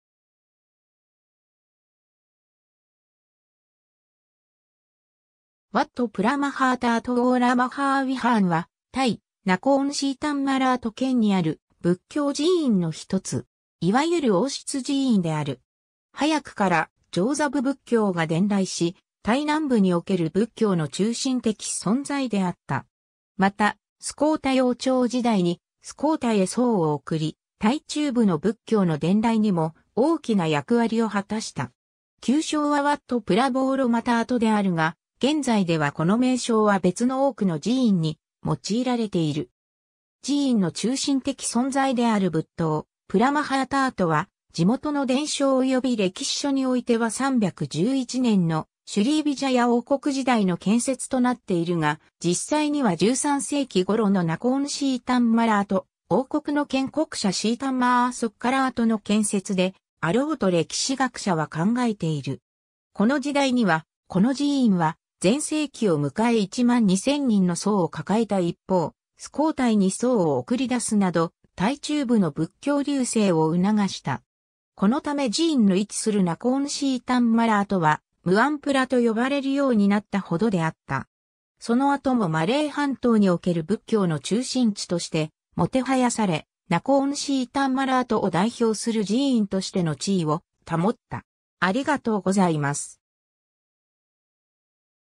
ワット・プラマハータート
ワット・プラマハータート＝ウォーラマハーウィハーンはタイ、ナコーンシータンマラート県にある仏教寺院の一つ。
いわゆる王室寺院である。
早くから上座部仏教（小乗仏教）が伝来しタイ南部における仏教の中心的存在であった。
またスコータイ王朝時代にスコータイへ僧を送り、タイ中部の仏教の伝来にも大きな役割を果たした。
旧称はワット・プラボーロマタートであるが、現在ではこの名称は別の多くの寺院に用いられている。
 寺院の中心的存在である仏塔、プラ・マハータートは地元の伝承および歴史書においては311年のシュリーヴィジャヤ王国時代の建設となっているが、実際には13世紀ごろのナコーンシータンマラート王国の建国者シータンマーソッカラートの建設であろうと歴史学者は考えている。
この時代には、この寺院は全盛期を迎え12,000人の僧を抱えた一方、スコータイに僧を送り出すなどタイ中部の仏教隆盛を促した。
このため寺院の位置するナコーンシータンマラートはムアンプラ（僧の街）と呼ばれるようになったほどであった。
その後もマレー半島における仏教の中心地としてもてはやされ、ナコーンシータンマラートを代表する寺院としての地位を保った。